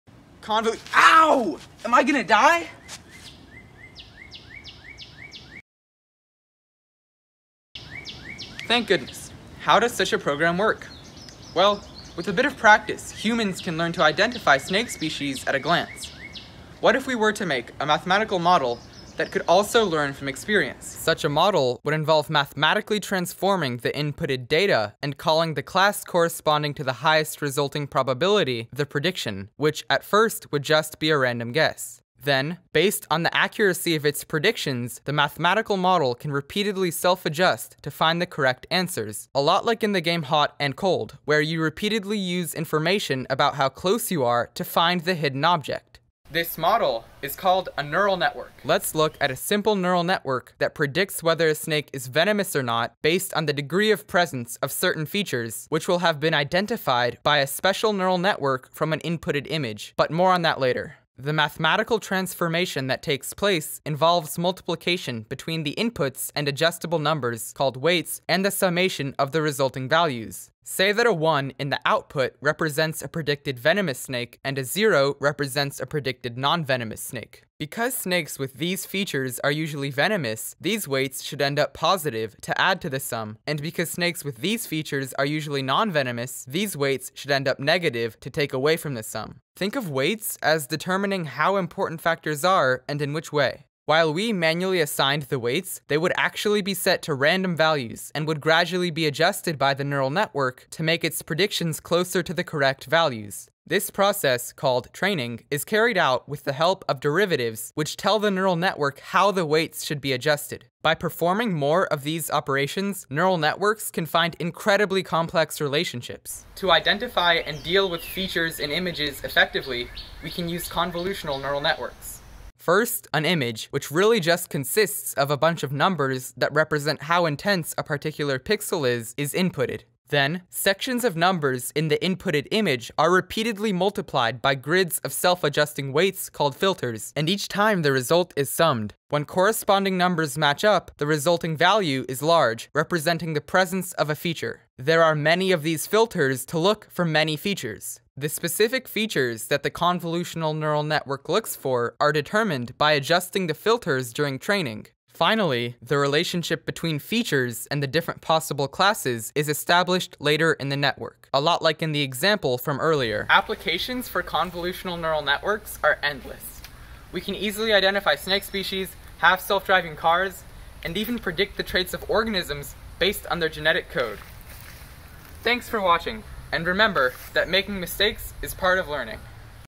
Convolutional Neural Networks Explained Simply in 3 Minutes | Breakthrough Junior Challenge 2021
This short video discusses convolutional neural networks with a fun application. This is my Breakthrough Junior Challenge 2021 submission.